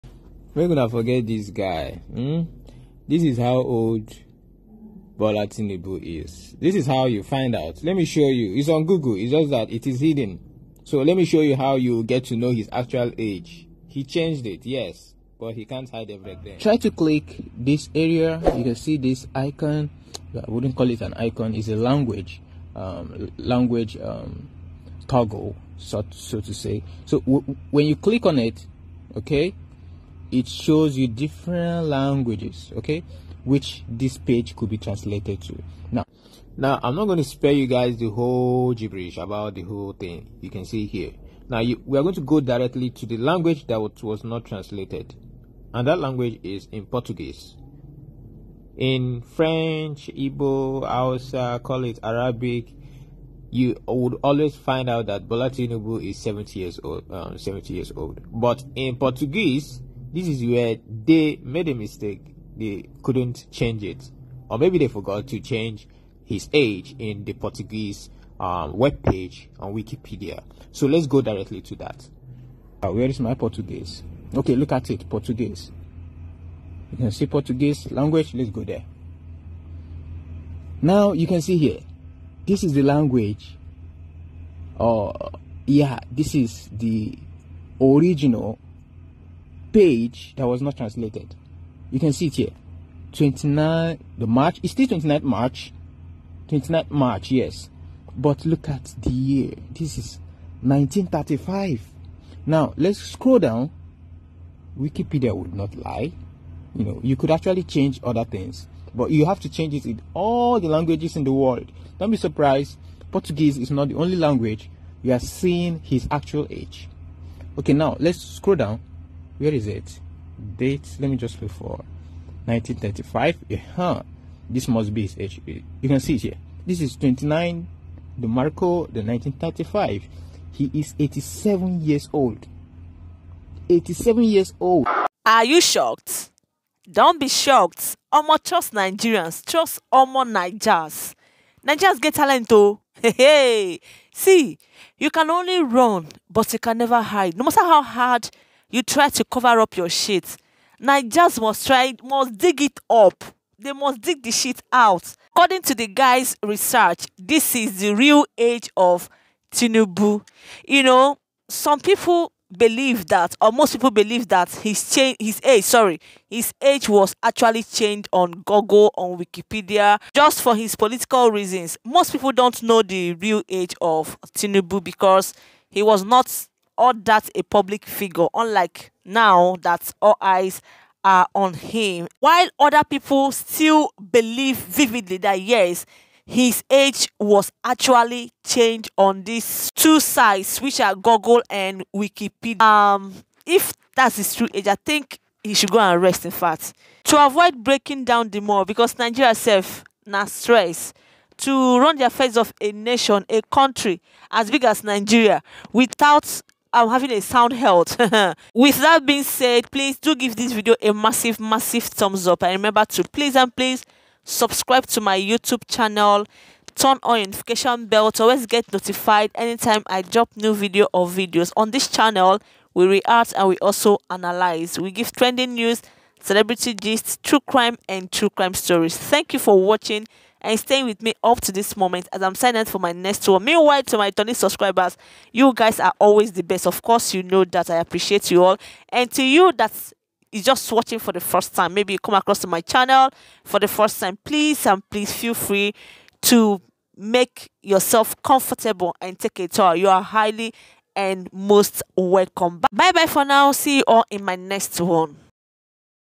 TINUBU Bursted😱🙊TINUBU Real Age Exposed Even If It Was Changed On GOOGLE
still on gistlover on tinubu series on gistloverblog:  tinubu real age revealed even if it was changed on google